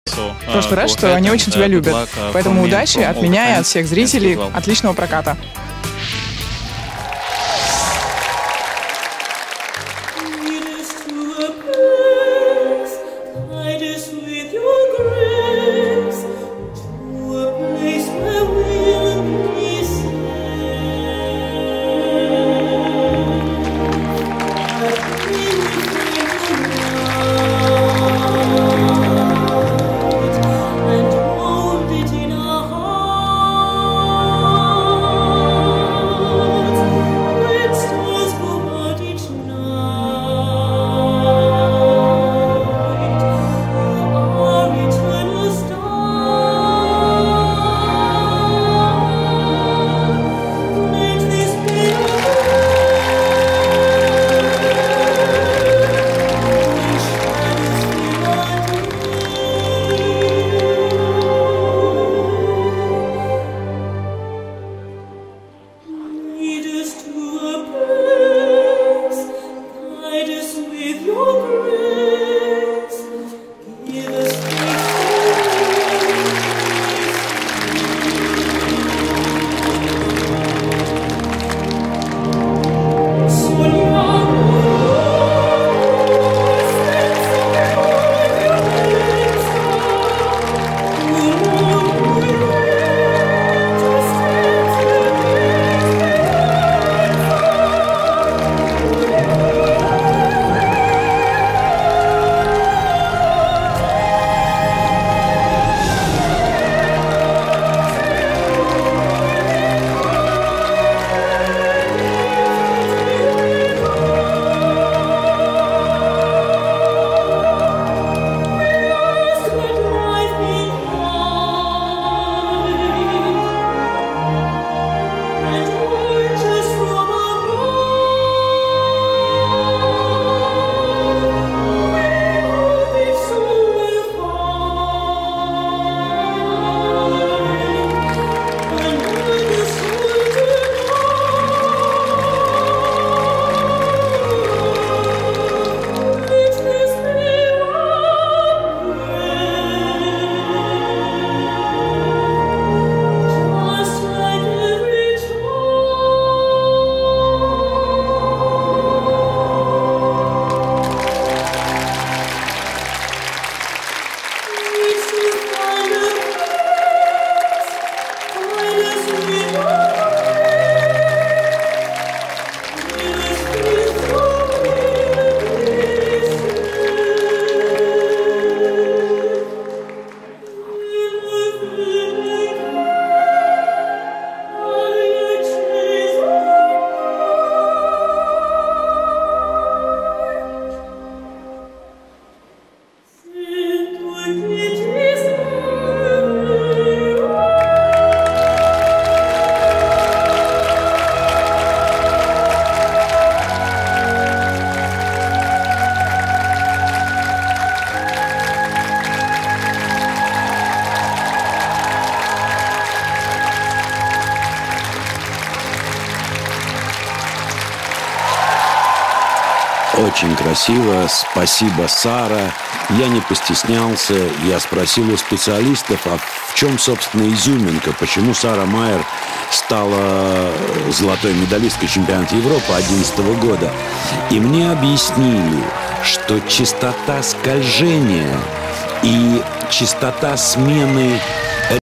Sarah Meier Ice Age Professional's Cup II 2014 01 19
ICE AGE 2014 - Professional's Cup II

Sarah Meier - Week 2 - Team Event Free Program